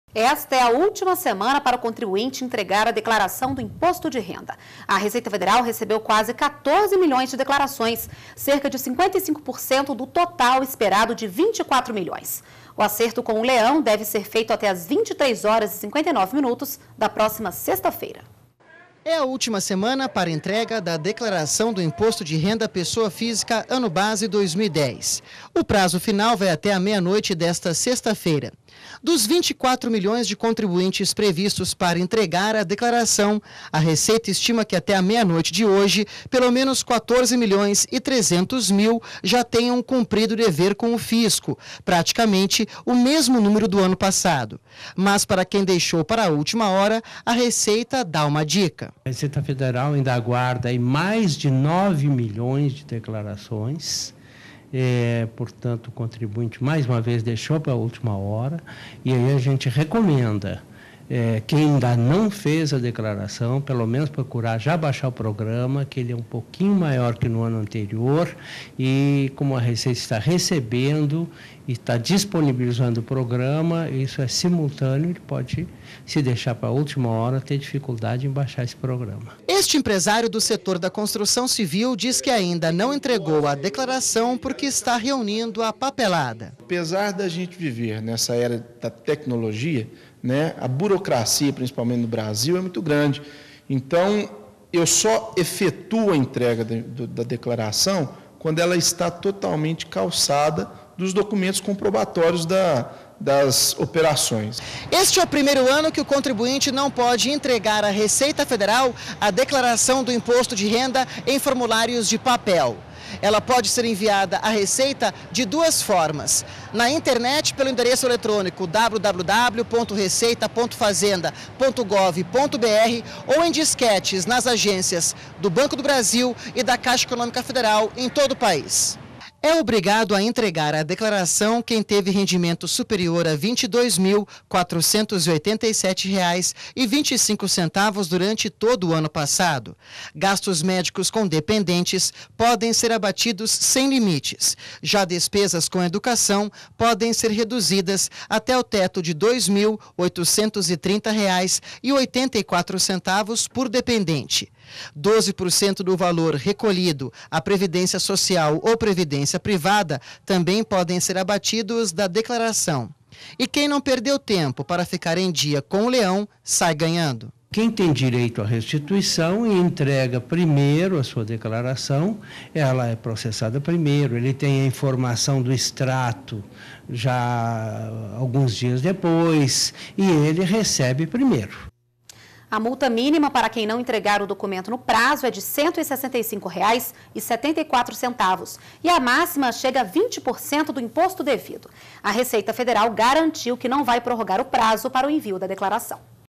Última semana para contribuinte entregar a declaração do Imposto de Renda
Esta é a última semana para o contribuinte entregar a declaração do Imposto de Renda. A Receita Federal recebeu quase 14 milhões de declarações, cerca de 55% do total esperado de 24 milhões. O acerto com o Leão deve ser feito até as 23 horas e 59 minutos da próxima sexta-feira dia 29/04/2011.